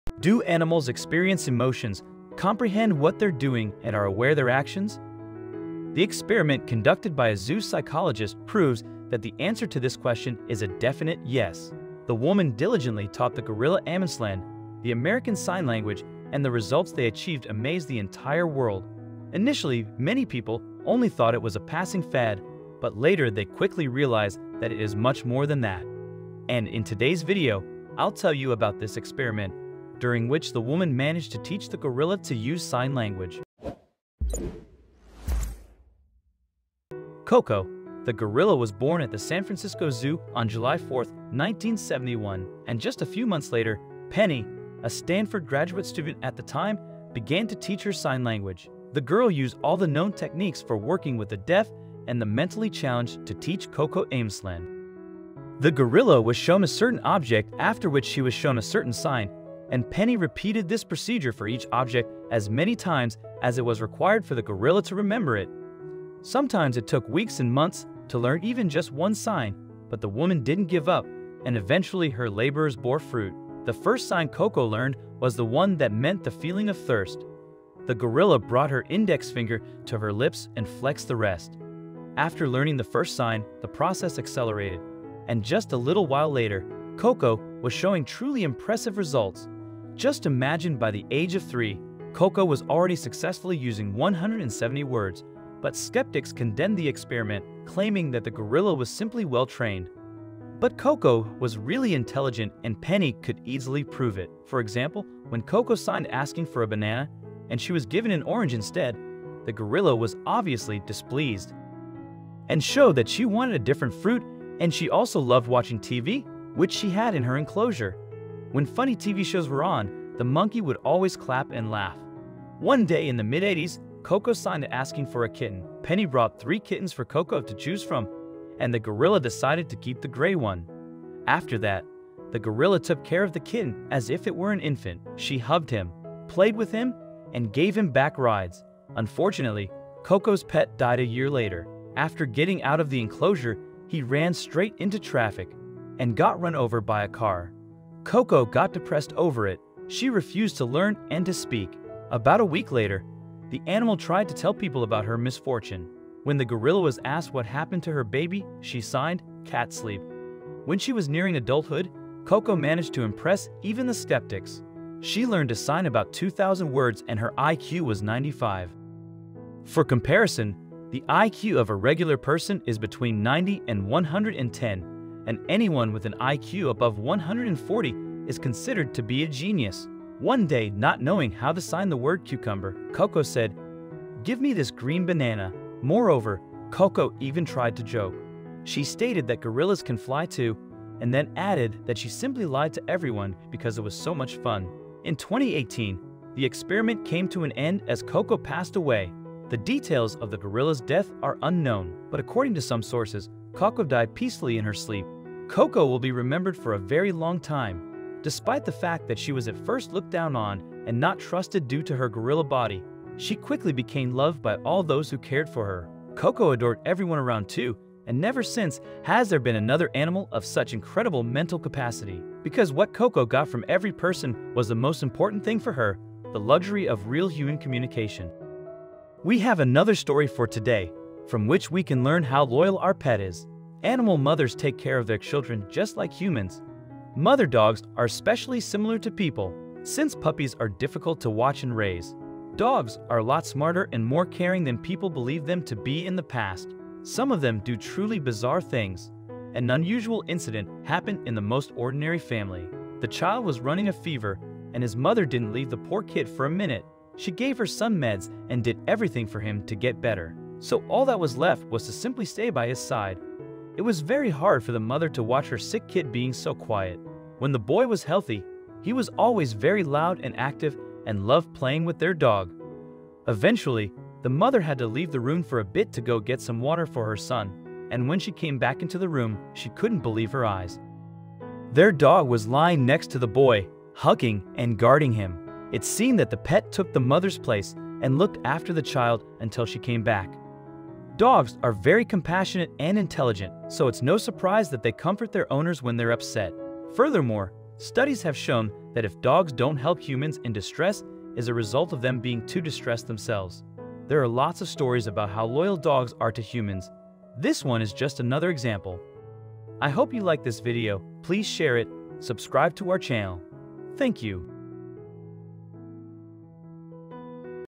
This woman spent 40 years teaching a gorilla to talk and the results are astounding
This woman spent 40 years teaching the gorilla to talk and what they achieved amazed the whole world